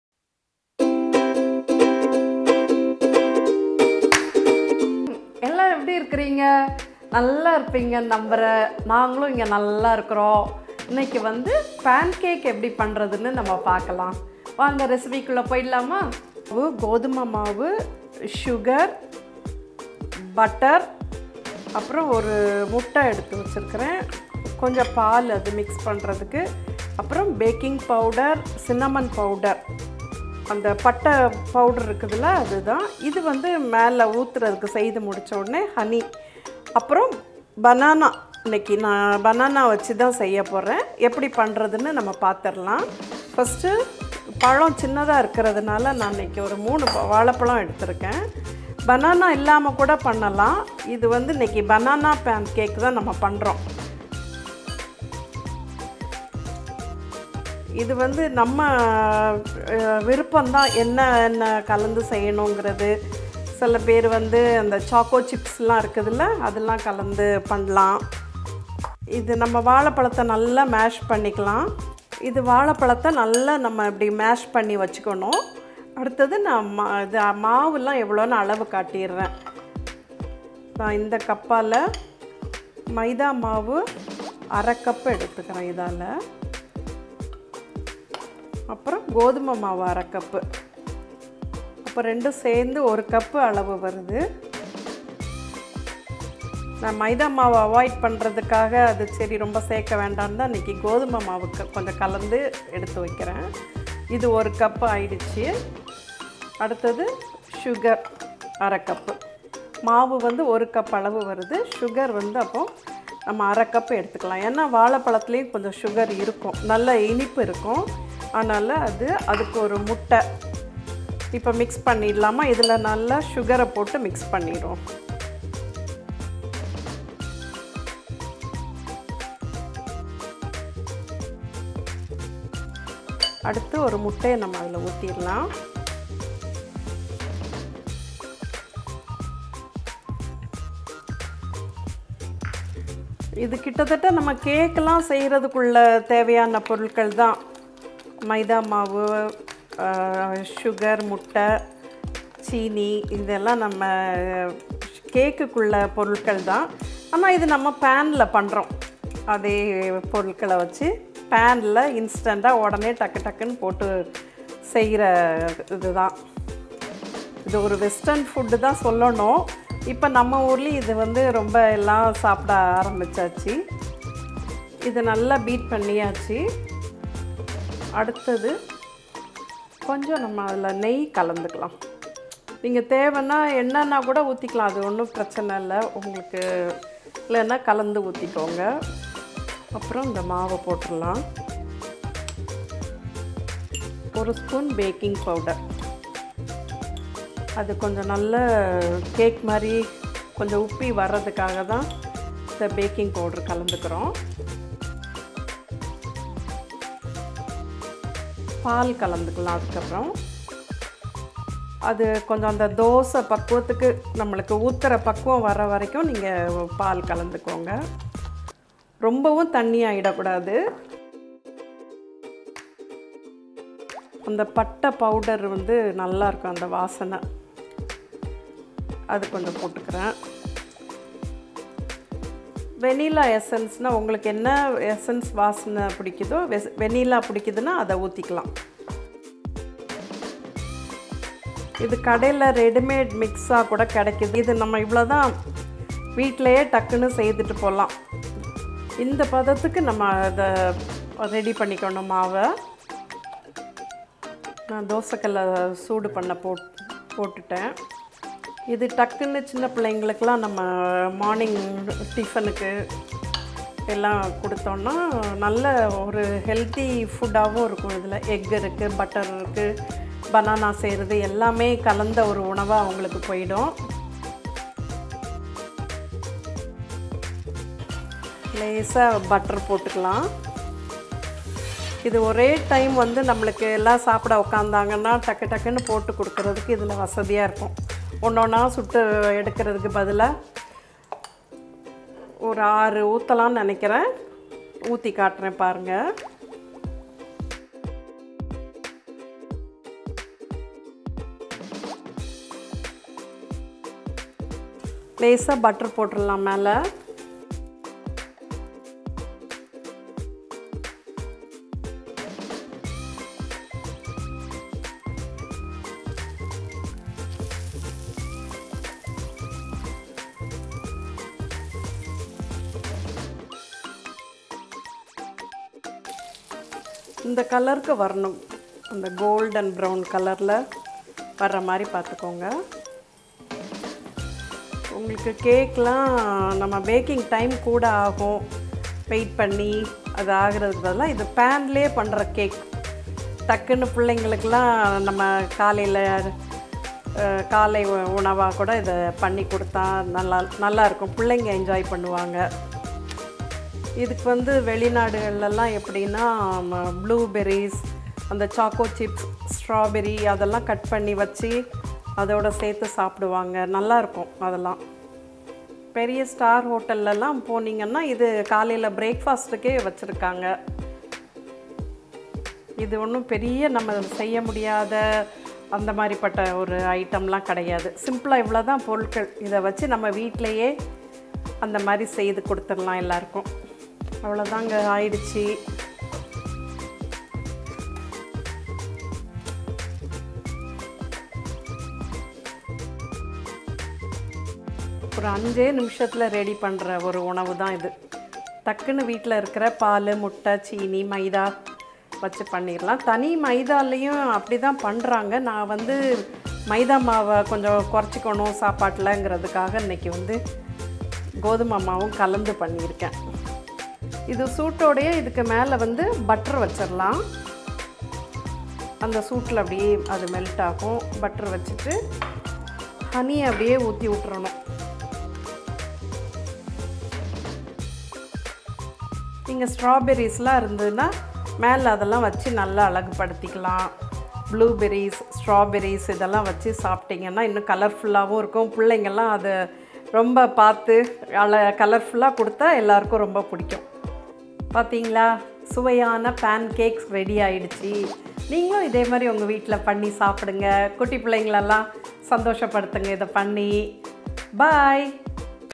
Banana Pancake Recipe | பான் கேக் செய்வது எப்படி | Easy cooking at home | kids favorite recipe
தேவையான பொருட்கள் :

வாழை பழம் - 3 nos
மைதா மாவு -1/2 cup
கோதுமை மாவு  -1/2 cup
அரைத்த சீனி - 1/2 cup
முட்டை - 1
பட்டை தூள் - 1 tsp
பேக்கிங் பவுடர் - 1 tsp
வெண்ணை - சிறிதளவு
 நெய்- சிறிதளவு
வெனிலா எசென்ஸ் - 1 tsp
பால் - தேவைக்கேற்ப்ப
தேன் - தேவைக்கேற்ப்ப